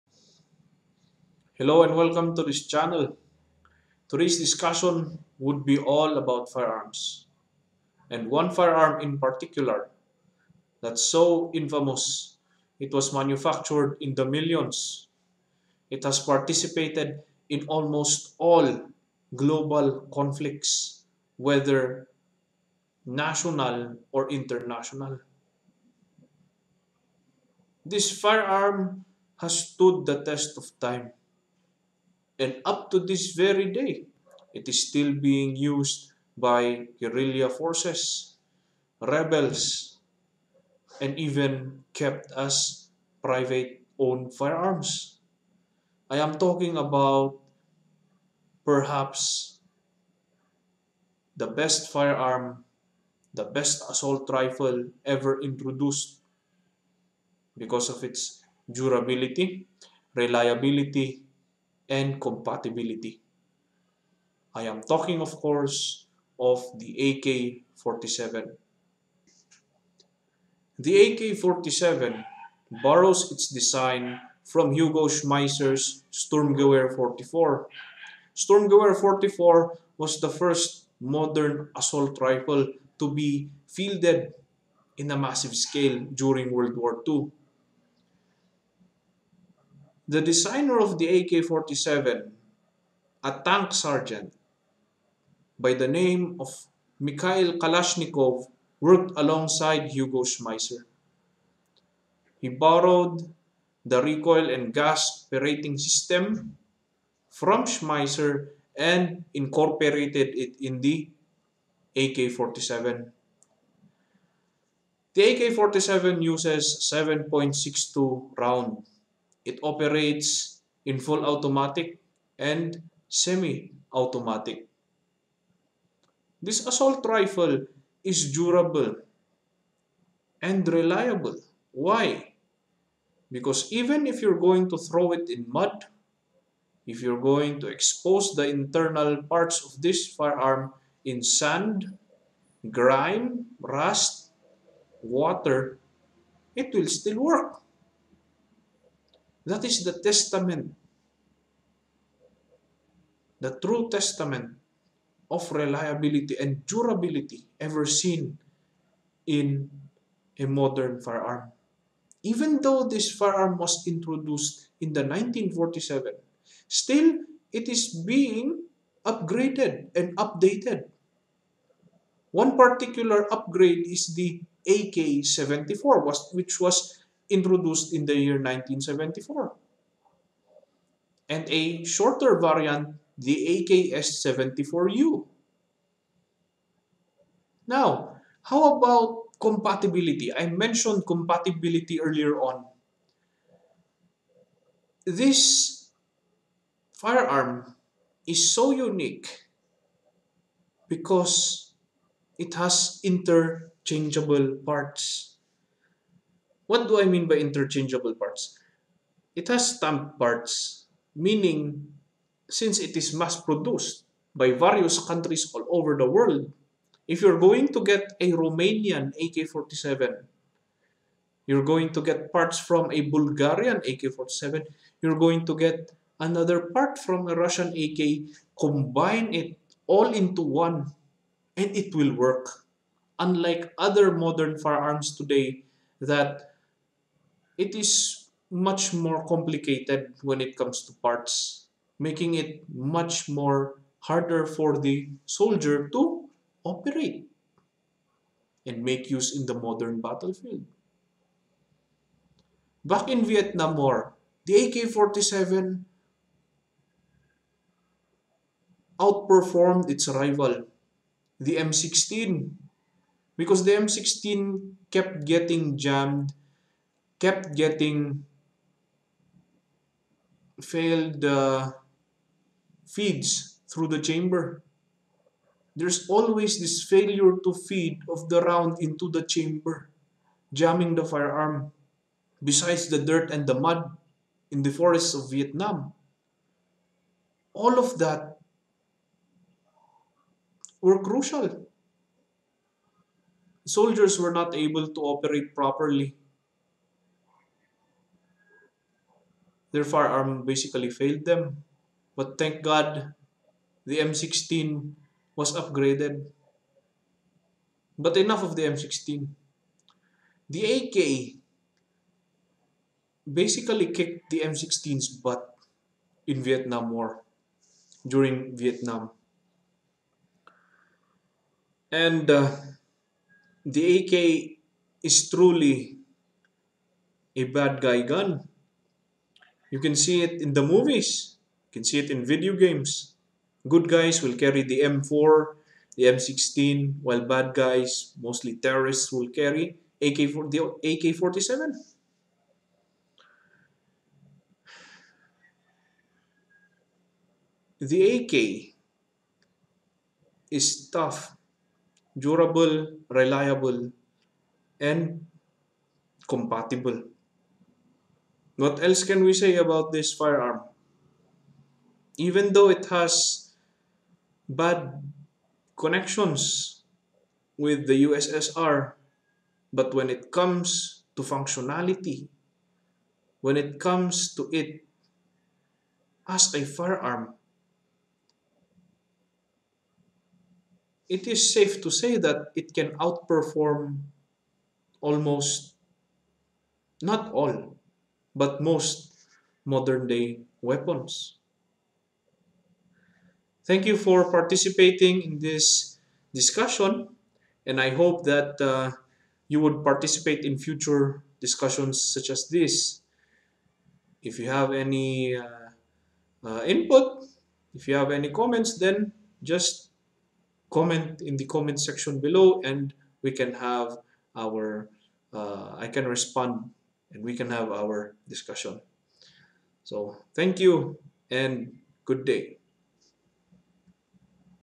FIREARMS: AK-47
Why is the AK-47 the best combat rifle?